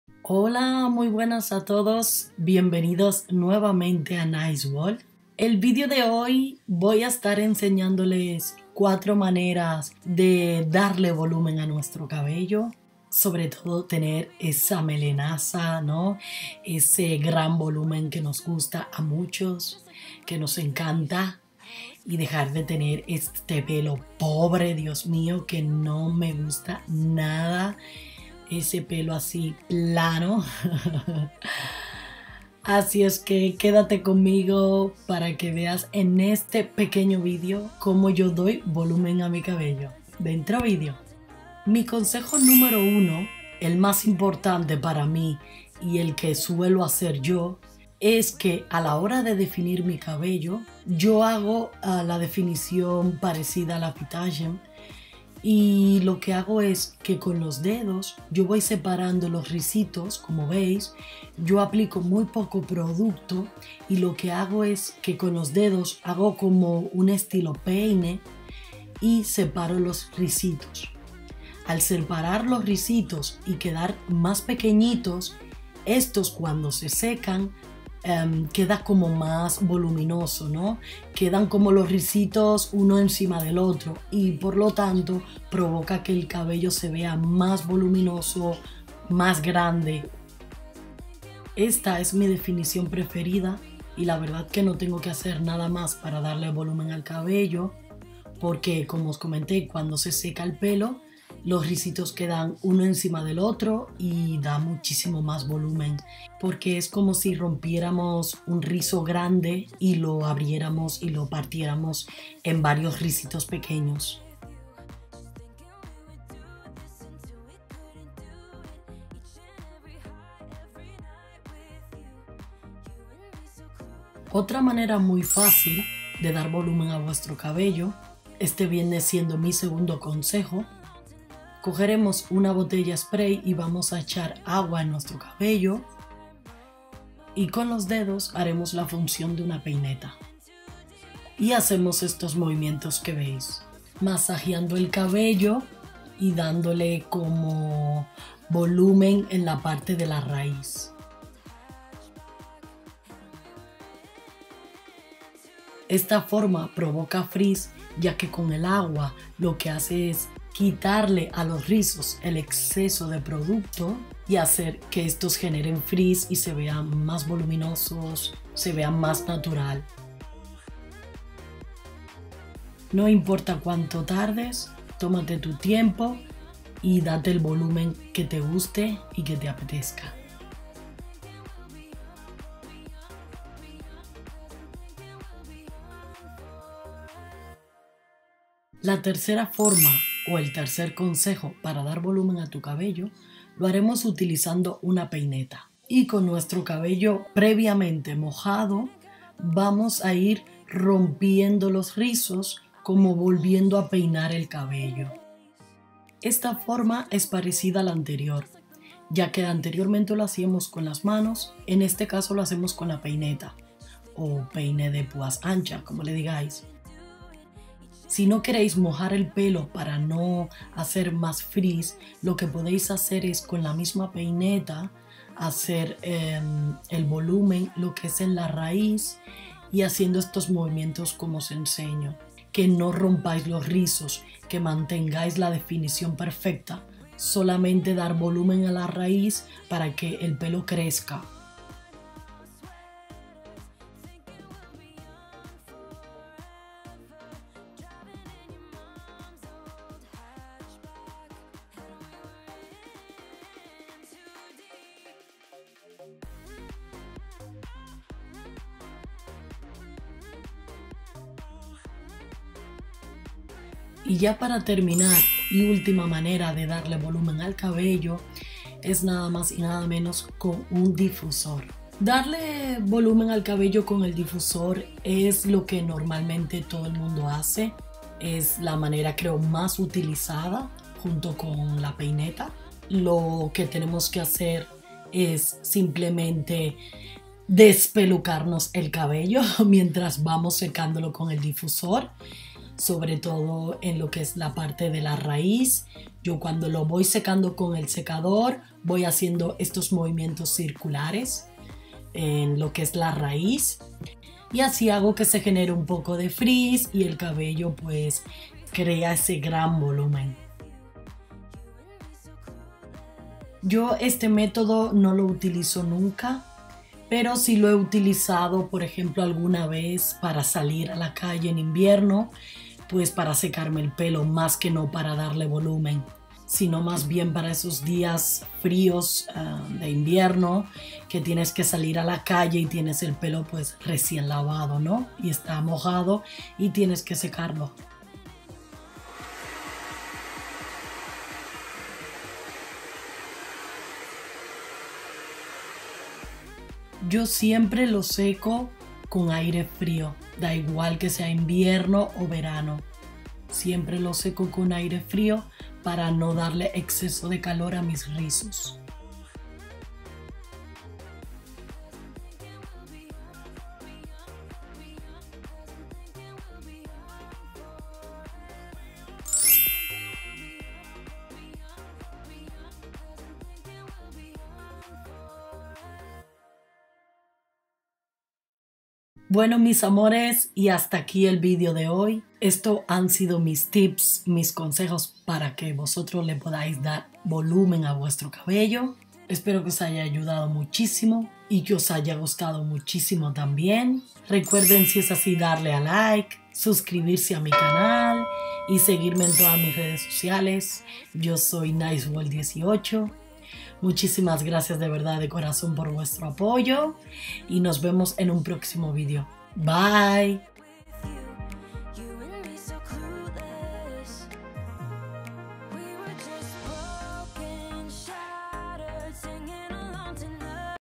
4 FORMAS DE DAR VOLUMEN A TU CABELLO
💖 Muy buenas a tod@s hermosuras 💖

En el vídeo de hoy les muestro 4 formas, sencillas, fáciles y espectaculares de dar VOLUMEN a tu CABELLO.

Espero que te sea de muchísima ayuda 👌🏽🤗

MUCHISIMAS GRACIAS.

Muaaa 💋

🟣 Mi equipo completo con el que grabo y edito todos los vídeos y fotos que publico: